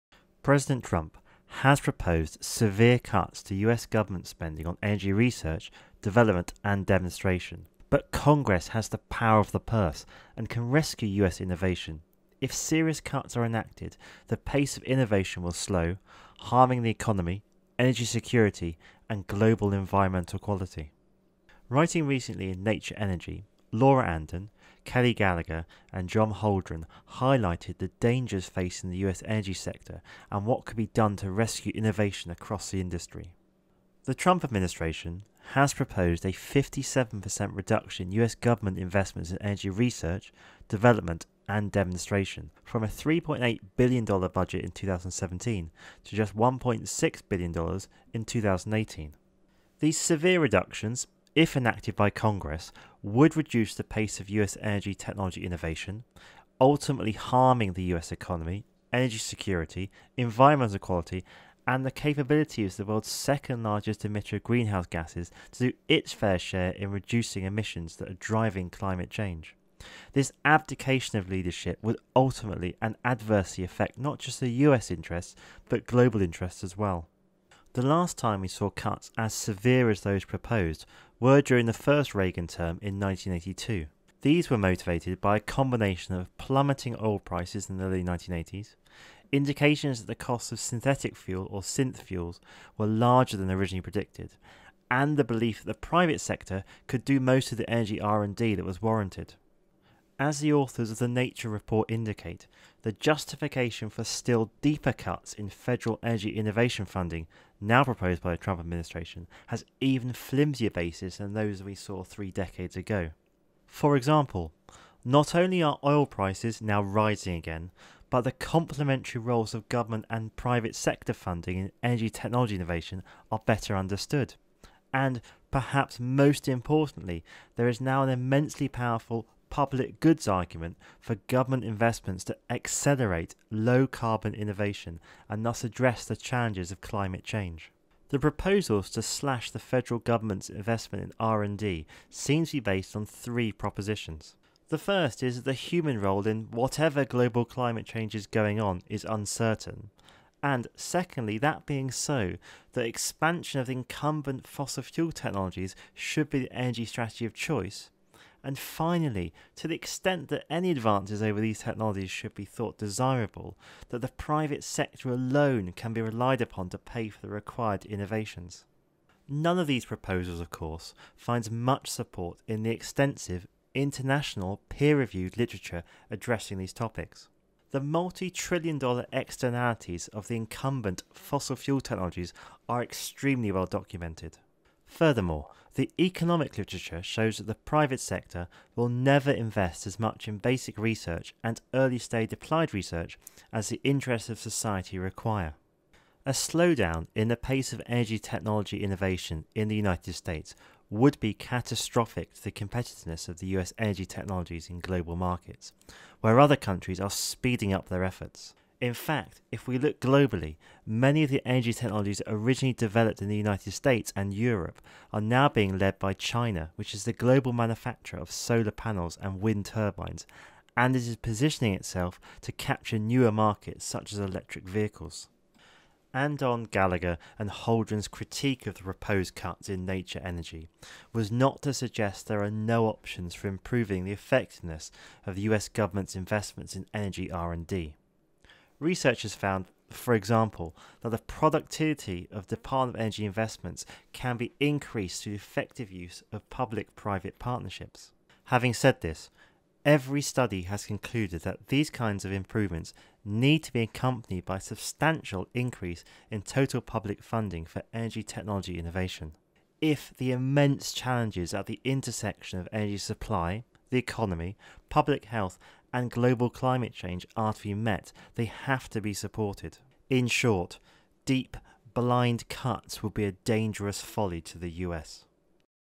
US Energy Innovation Under Threat
President Trump has proposed severe cuts to US government spending on energy research, development and demonstration. If serious cuts are enacted, the pace of innovation will slow, harming the economy, energy security and global environmental quality.

References

- Rescue US energy innovation - Nature Energy
- FY18 R&D Appropriations Dashboard : R&D Budget and Policy Program (AAAS, 2017);
- President’s Council of Advisors on Science and Technology. Federal Energy R&D for the Challenges of the 21st Century (Executive Office of the President of the United States, Washington DC, 1997);
- Greenfieldboyce, N. Trump’s budget slashes climate change funding. The Two-Way (16 March 2017);